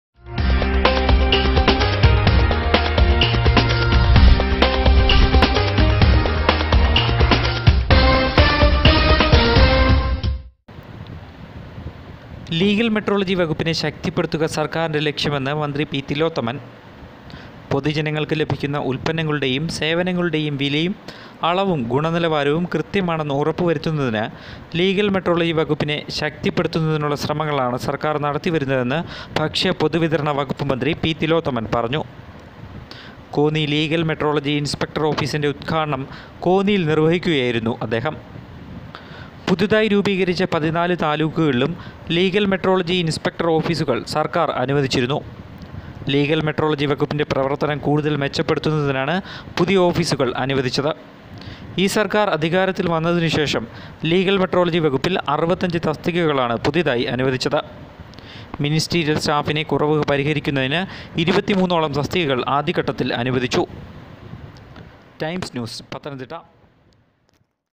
ലീഗല് മെട്രോളജിയെ ശക്തിപ്പെടുത്തുമെന്നു മന്ത്റി പി തിലോത്തമന്
inaguration Pathanamthitta legal metrolgy inspector office